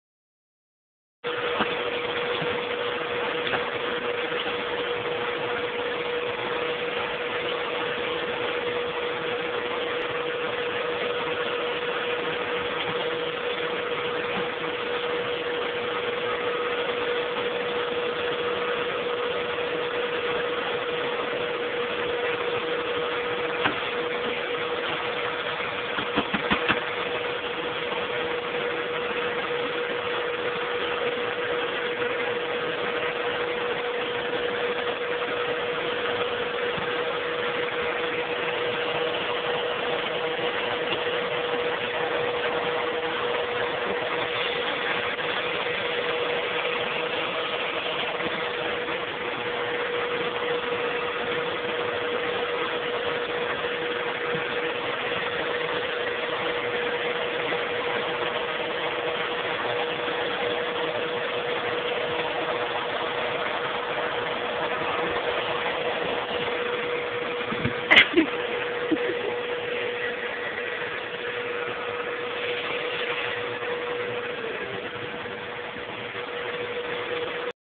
Cat Fighting Vacuum (Very Funny!)
My Cat always gets freaked out by the vacuum. He hates it's loud sound, but this time he attempted to fight it off by taking a couple swats at the appliance! LOL!